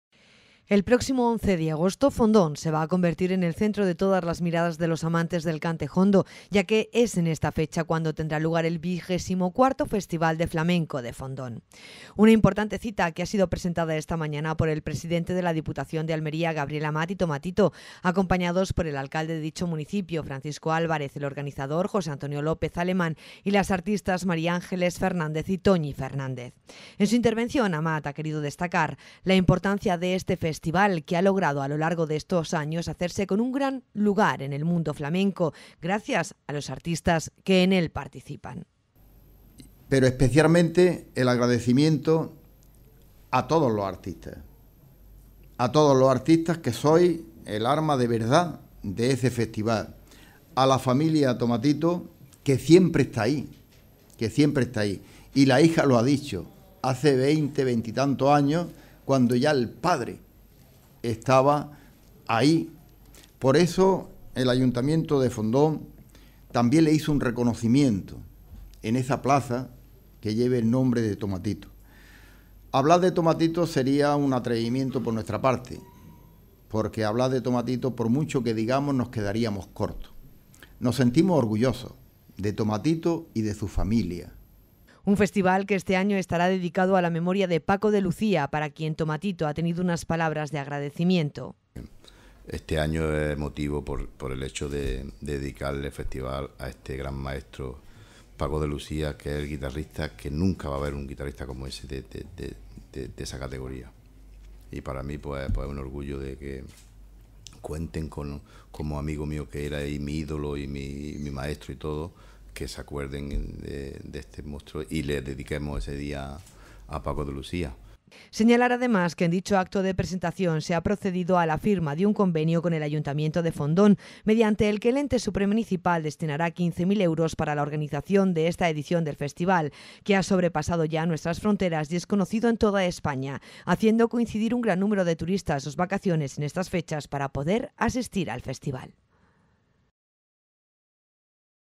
Almería Noticias Digital 28 TV - Diputación presenta el XXIV Festival de Flamenco de Fondón
El próximo 11 de agosto Fondón se va a convertir en el centro de todas las miradas de los amantes del cante “jondo” ya que es en esta fecha cuando tendrá lugar el XXIV Festival de Flamenco de Fondón.